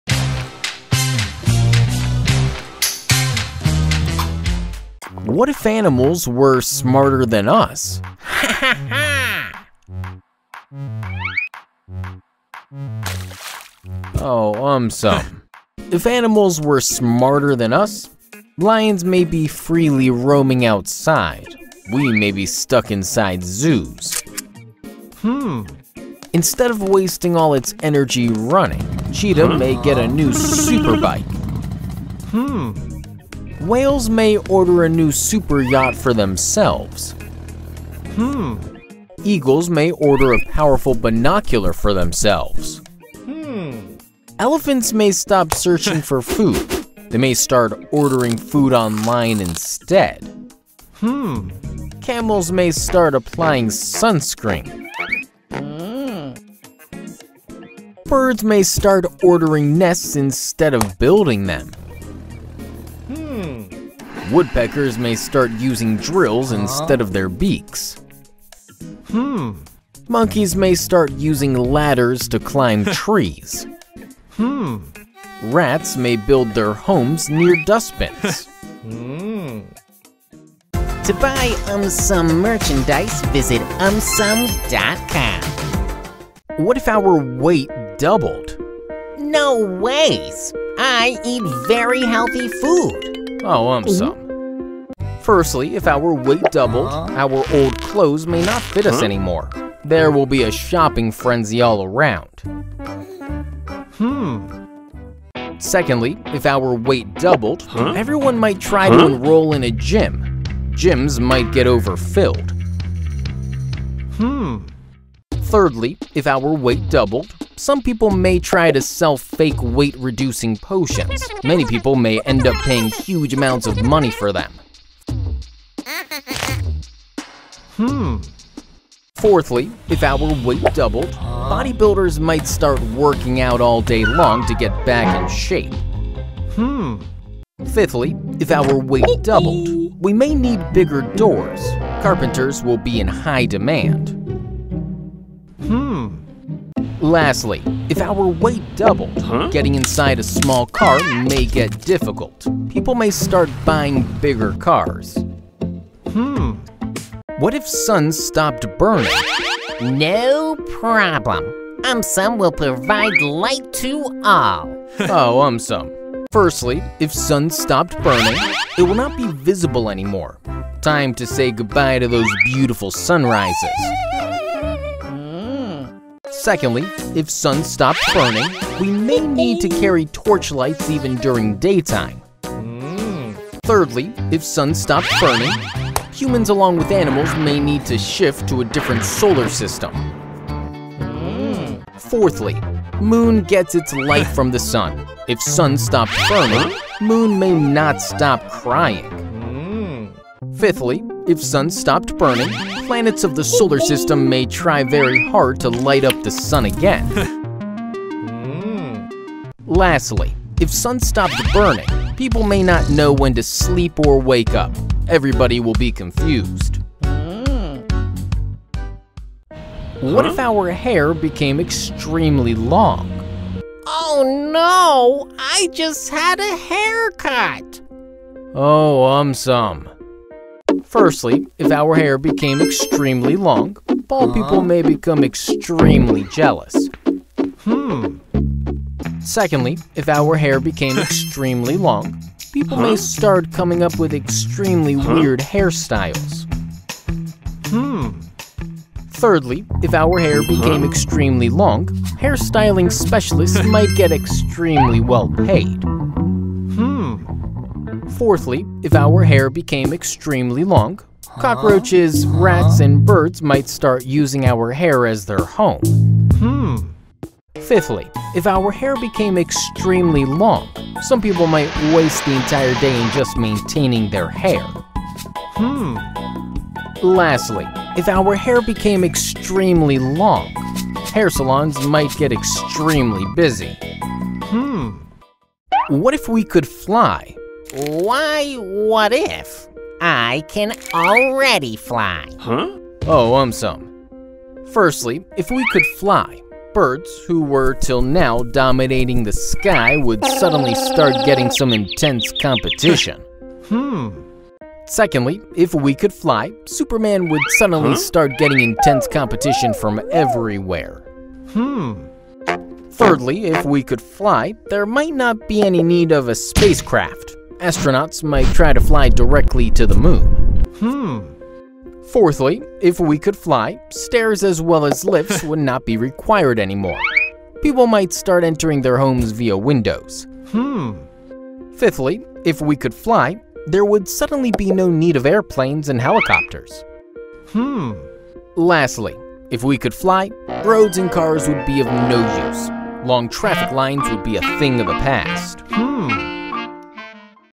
What if Animals were Smarter than us? + more videos
If Animals were Smarter than us:
Lions may be freely roaming outside, we may be stuck inside zoos.
Instead of wasting all its energy running, Cheetah may get a new super-bike.
Whales may order a new super yacht for themselves.
Eagles may order a powerful binocular for themselves.
Elephants may stop searching for food, they may start ordering food online instead.
Camels may start applying sunscreen.
Birds may start ordering nests instead of building them.
Woodpeckers may start using drills instead of their beaks.
Monkeys may start using ladders to climb trees.
Rats may build their homes near dustbins.

Timecodes
0:00 - What if Animals were Smarter than us?
1:26 - What if our Weight Doubled?
2:37 - What if Sun Stopped Burning?
3:39 - What if our Hair became extremely Long ?
4:45 - What if we could Fly?